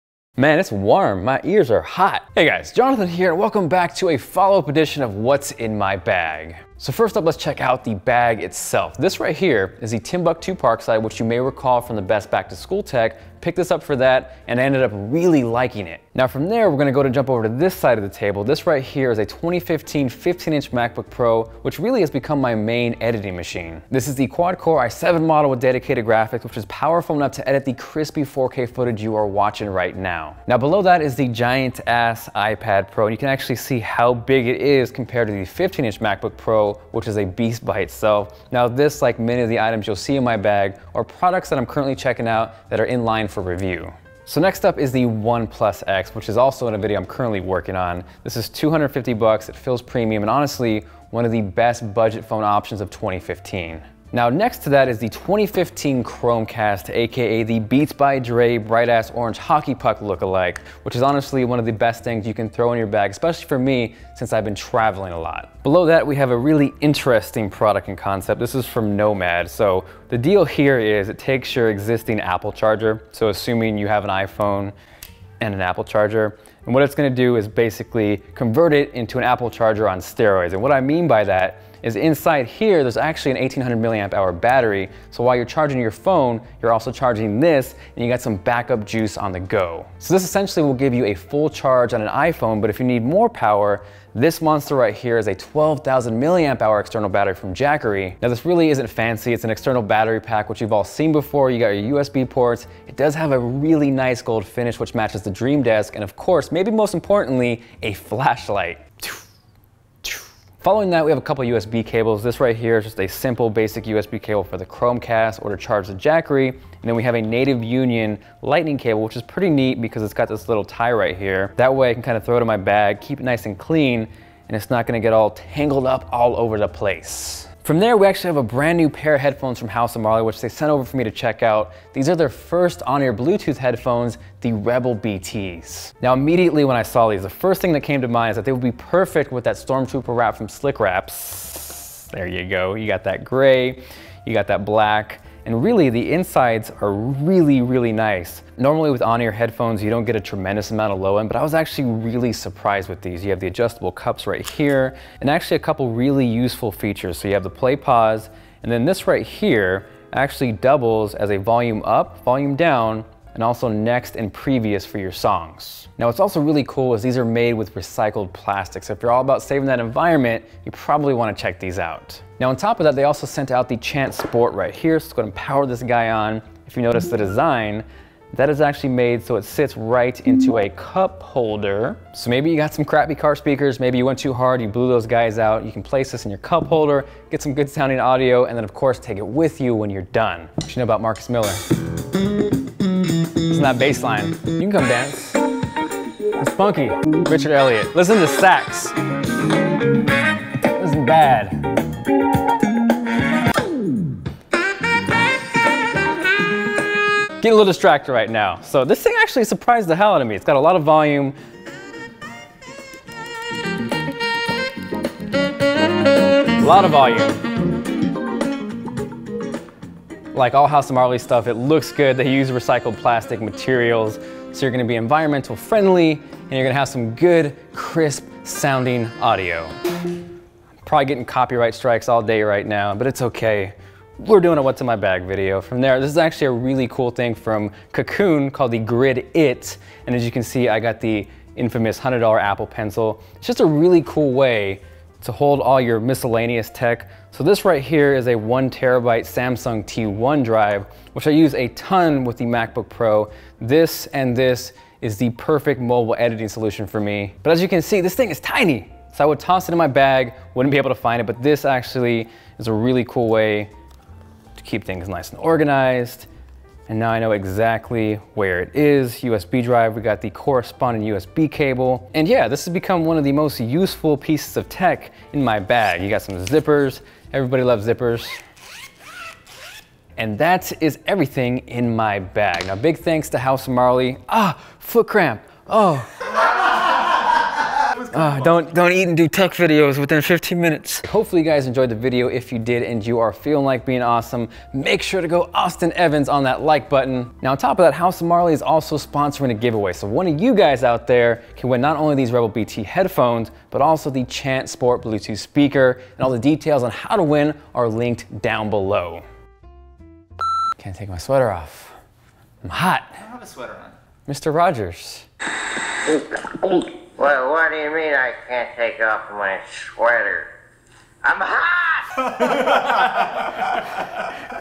What's in My Tech Bag! v2.0
What's in My Tech Bag for 2015. Enjoy! :)

Grid-It Organizer
Samsung T1 1TB SSD
15" Retina MacBook Pro
iPad Pro
Apple Pencil
Native Union Lightning Cable
Jackery Giant+ 12,000 mAh Battery
Nomad Apple Charger